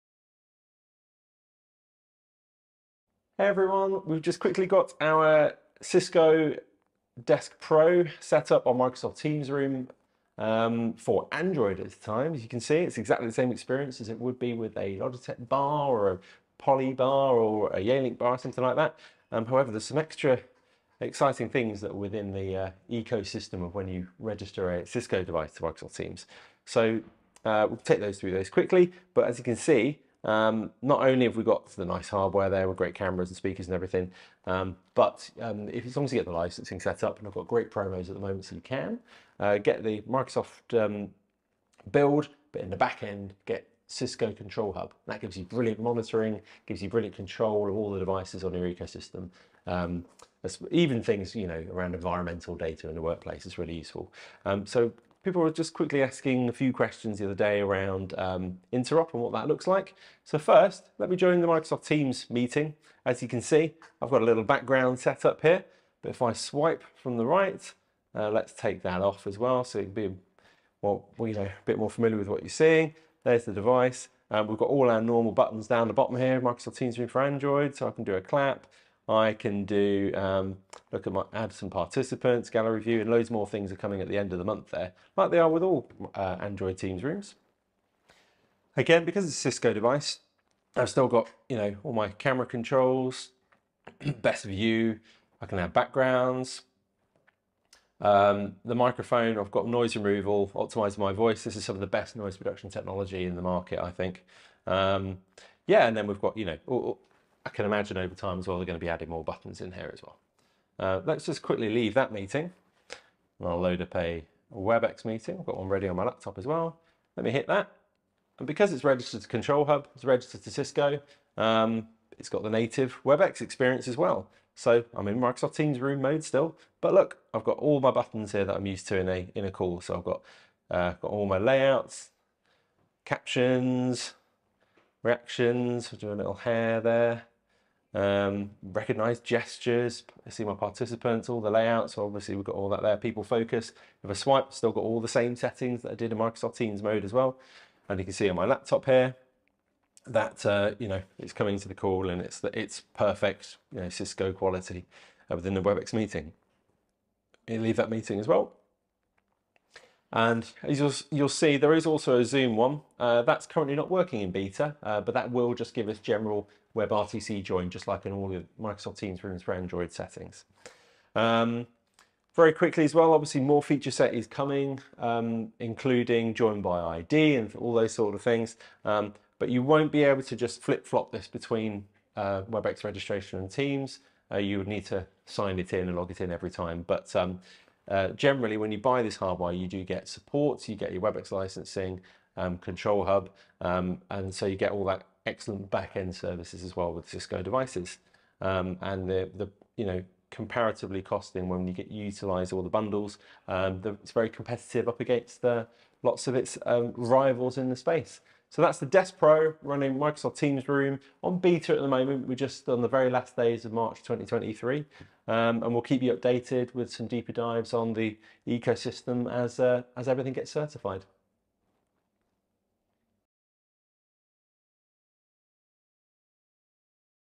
Cisco Microsoft Teams Rooms Beta Demo March 2023
0:00 – Intro
00:18 – Introduction to the device on android
00:43 – Cisco control hub
01:00 – Microsoft teams meeting
01:40 – Key features
02:00 – Cisco meeting and features

Any questions please reach out on: isdmsolutions.com